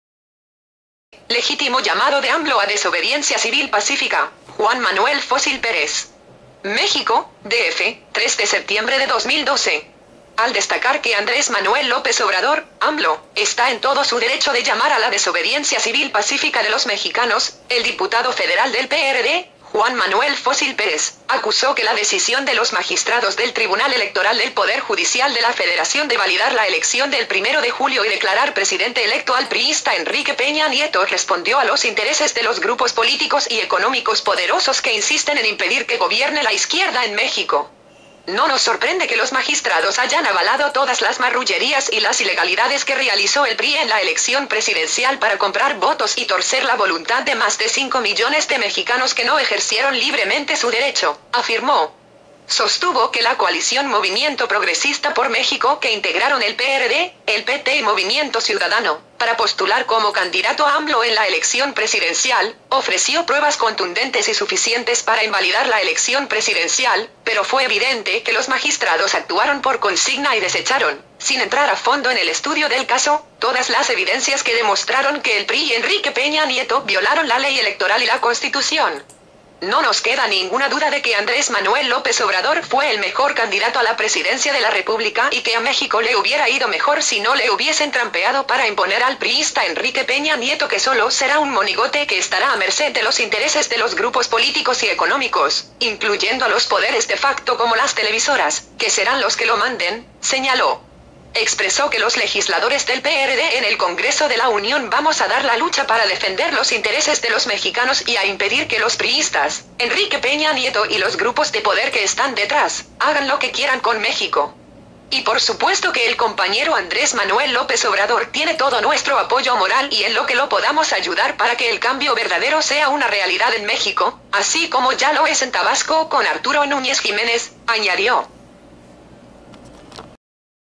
Legítimo llamado de AMLO a desobediencia civil pacífica, Juan Manuel Fócil Pérez.mpg
Legítimo llamado de AMLO a desobediencia civil pacífica, Juan Manuel Fócil Pérez.

MÉXICO, DF, 03 DE SEPTIEMBRE DE 2012.- Al destacar que Andrés Manuel López Obrador (AMLO) está en todo su derecho de llamar a la desobediencia civil pacífica de los mexicanos, el diputado federal del PRD, Juan Manuel Fócil Pérez, acusó que la decisión de los magistrados del Tribunal Electoral del Poder Judicial de la Federación de validar la elección del primero de julio y declarar Presidente electo al priista Enrique Peña Nieto respondió a los intereses de los grupos políticos y económicos poderosos que insisten en impedir que gobierne la izquierda en México.
"No nos sorprende que los magistrados hayan avalado todas las marrullerías y las ilegalidades que realizó el PRI en la elección presidencial para comprar votos y torcer la voluntad de más de 5 millones de mexicanos que no ejercieron libremente su derecho", afirmó.
Sostuvo que la Coalición Movimiento Progresista por México que integraron el PRD, el PT y Movimiento Ciudadano, para postular como candidato a AMLO en la elección presidencial, ofreció pruebas contundentes y suficientes para invalidar la elección presidencial, "pero fue evidente que los magistrados actuaron por consigna y desecharon, sin entrar a fondo en el estudio del caso, todas las evidencias que demostraron que el PRI y Enrique Peña Nieto violaron la ley electoral y la Constitución".
"No nos queda ninguna duda de que Andrés Manuel López Obrador fue el mejor candidato a la Presidencia de la República y que a México le hubiera ido mejor si no le hubiesen trampeado para imponer al priista Enrique Peña Nieto que solo será un monigote que estará a merced de los intereses de los grupos políticos y económicos, incluyendo a los poderes de facto como las televisoras, que serán los que lo manden", señaló.
Expresó que los legisladores del PRD en el Congreso de la Unión "vamos a dar la lucha para defender los intereses de los mexicanos y a impedir que los priistas, Enrique Peña Nieto y los grupos de poder que están detrás, hagan lo que quieran con México".
"Y por supuesto que el compañero Andrés Manuel López Obrador tiene todo nuestro apoyo moral y en lo que lo podamos ayudar para que el cambio Verdadero sea una realidad en México, así como ya lo es en Tabasco con Arturo Núñez Jiménez", añadió.